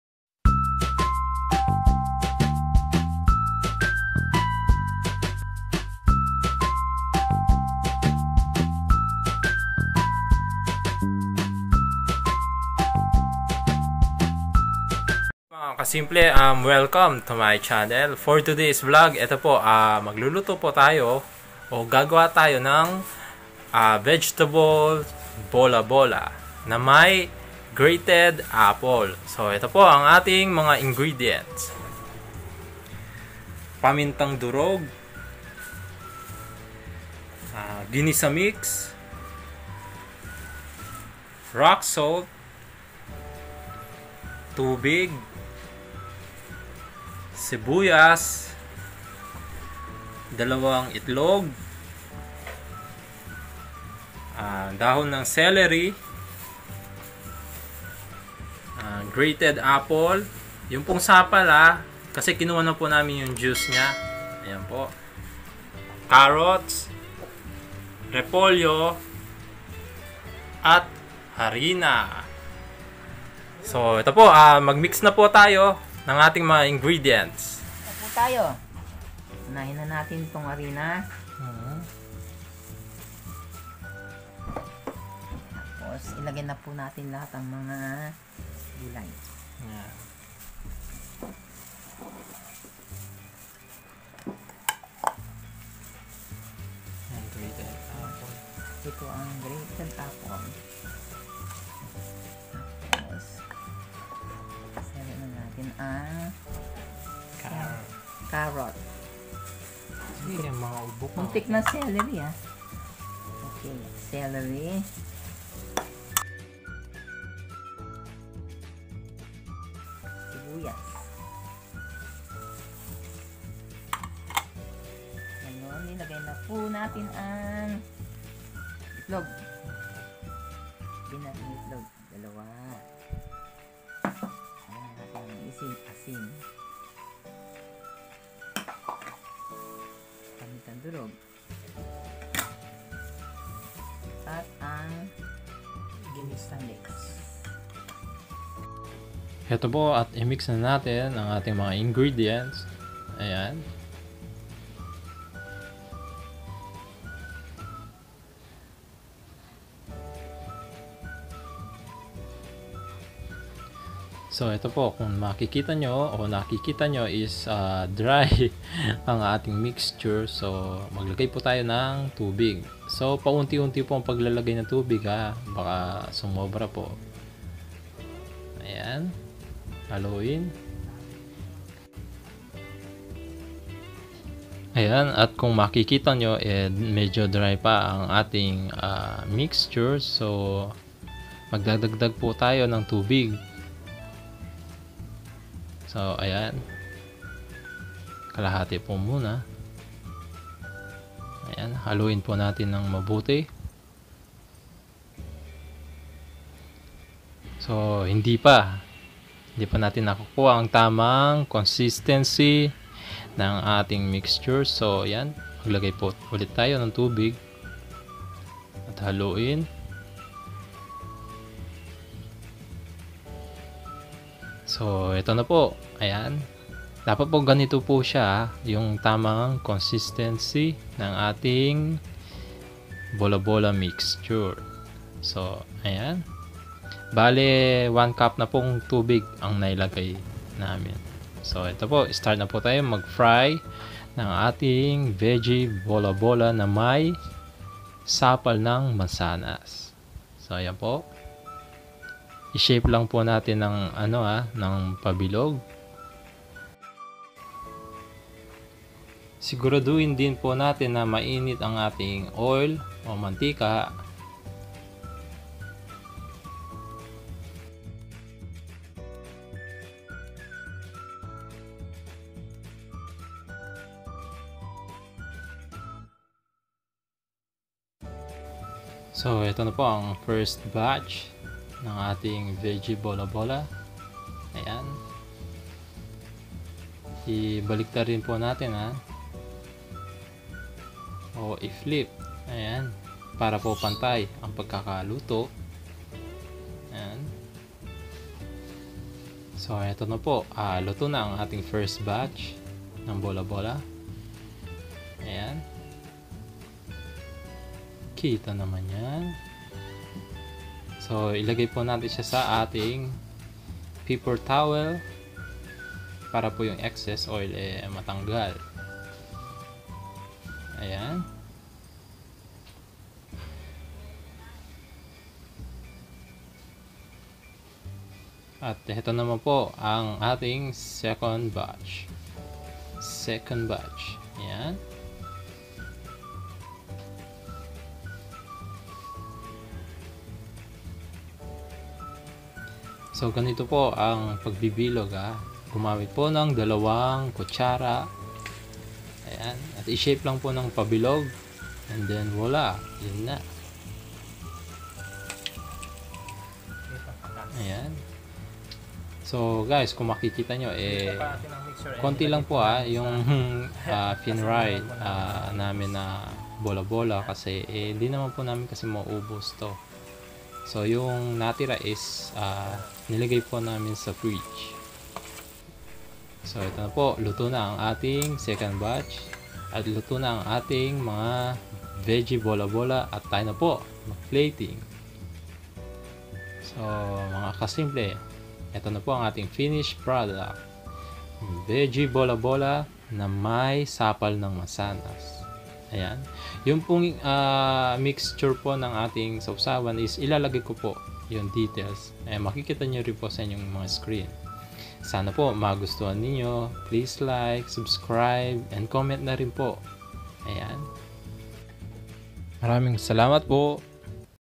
VEGGIE BOLA BOLA | VEGGIE BOLA BOLA RECIPE | VEGGIE BOLA BOLA NA MAY SAPAL NG MANSANAS.
VEGGIE BOLA BOLA

Ingredients

Pamintang Durog/Black Pepper Powder
Ginisa Mix
Asin/Sea Salt
Tubig/Water
Sibuyas/Onion
2 Binating Itlog/Beaten Eggs
Celery
Sapal ng Mansanas/Apple Pulp
Carrots
Repolyo/Cabbage
Harina/Flour